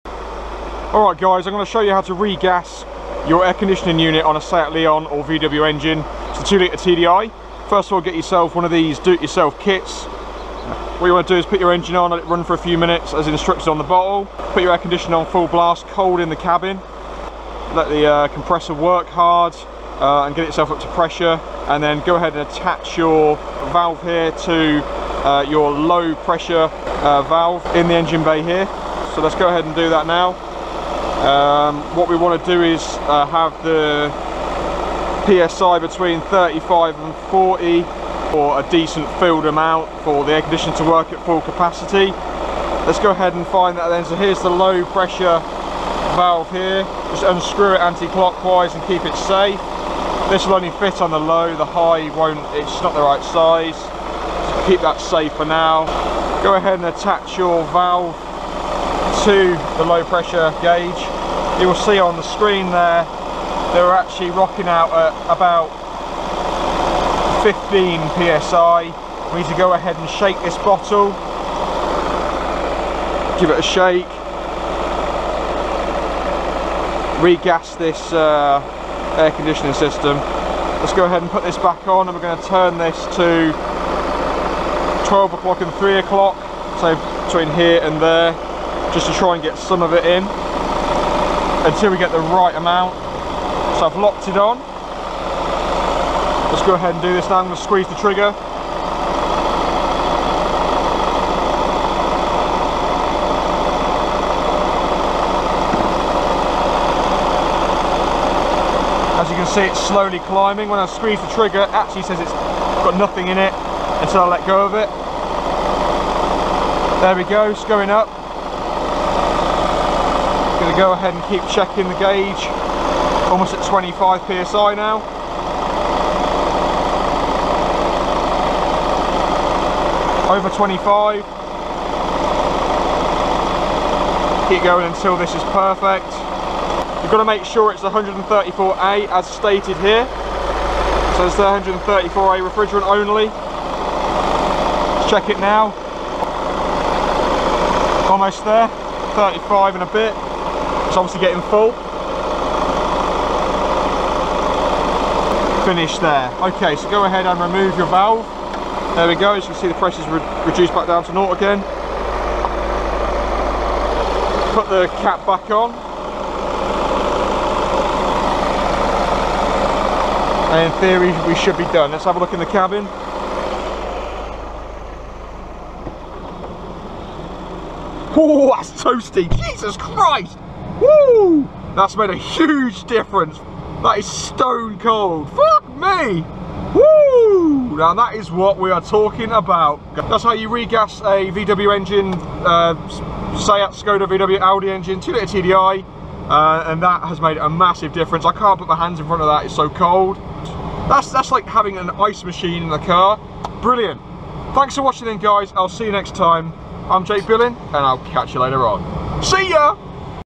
How to Recharge a Car Air Conditioning System SEAT LEON TDI VW/AUDI/SKODA/SEAT
This video shows how to recharge (regas) a car's air conditioning system to make it work correctly. If you notice your AC isn't as cold as it used to be then it probably needs recharging.

This video applies to all VW/AUDI/SEAT & Skoda's. TDI.